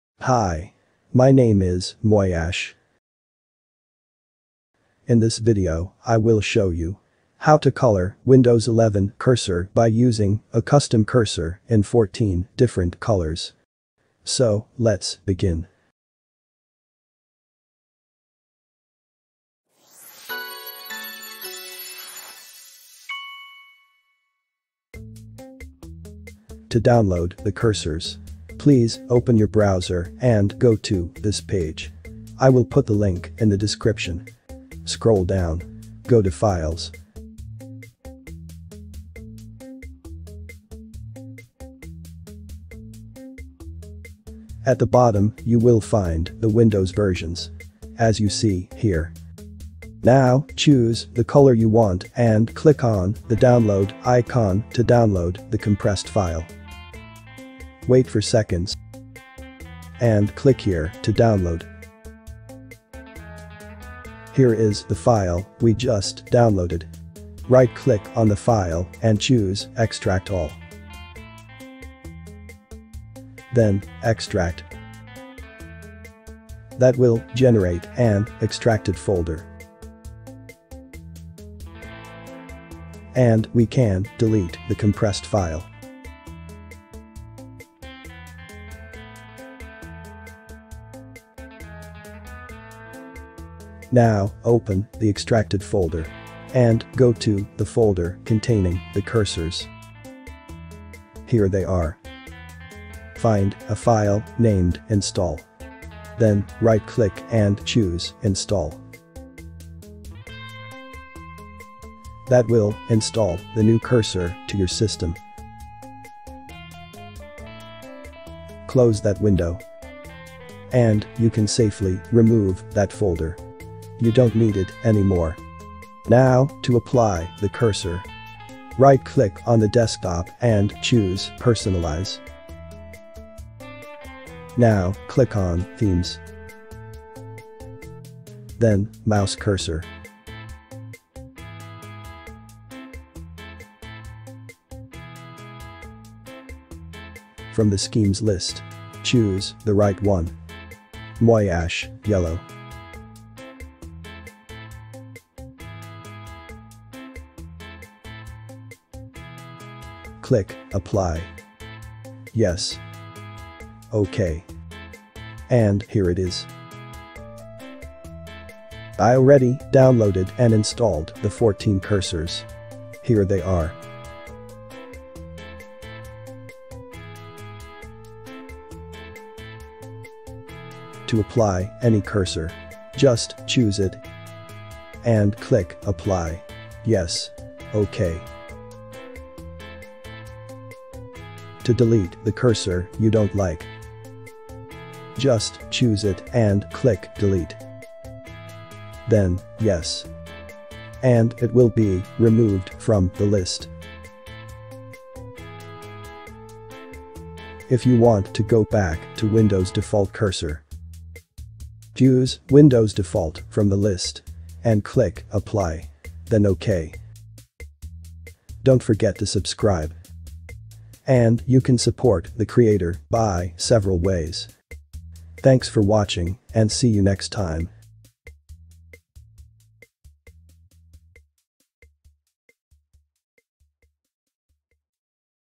Change Your Mouse Cursor Color in Windows 11
How to Change Your Mouse Cursor Color in Windows 11 by Using Custom Cursors